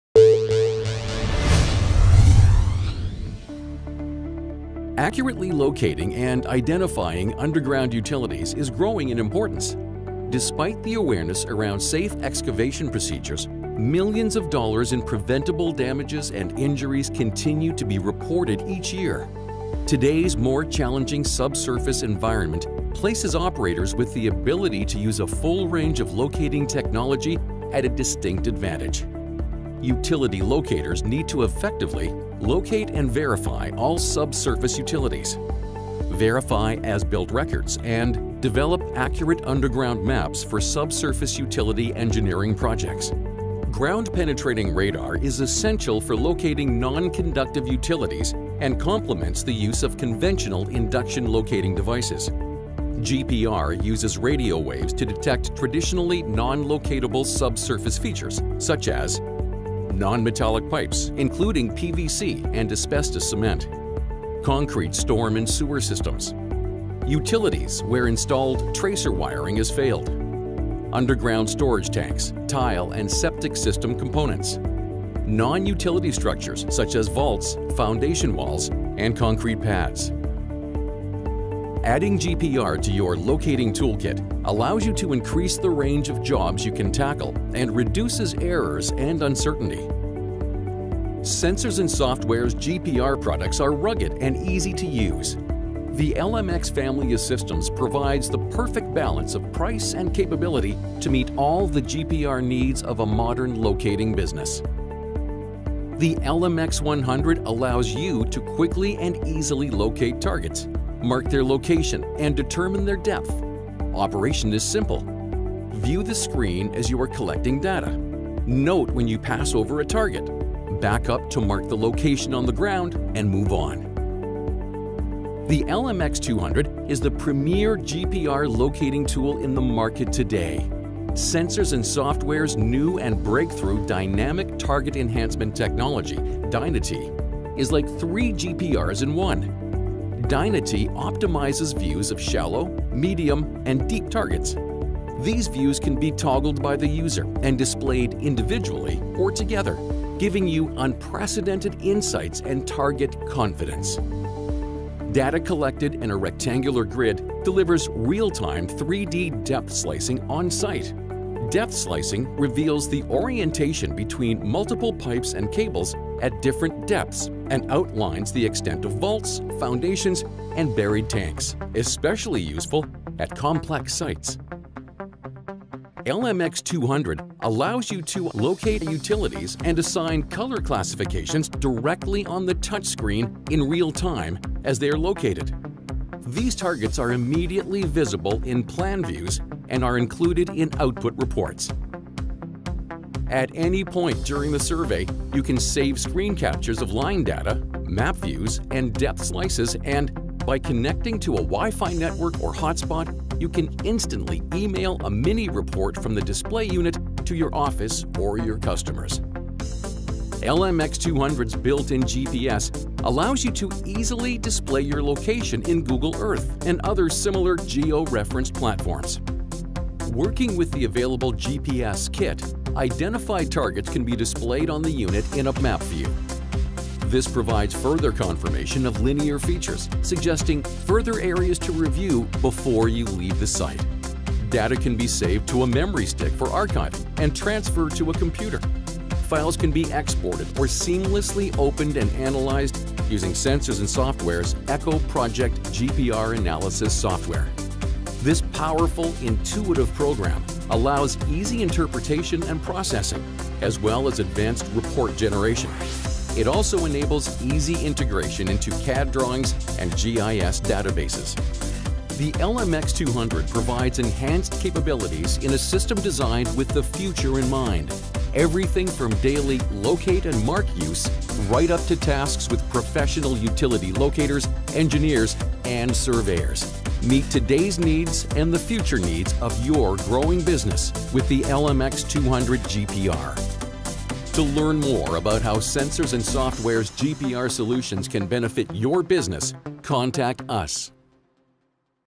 Phil Curran narration for ground penetrating radar supplier Sensors & Software.
Featuring voice over by Phil Curran, this video for ground penetrating radar manufacturer Sensors & Software highlights the premier GPR locating tool available, the LMX200.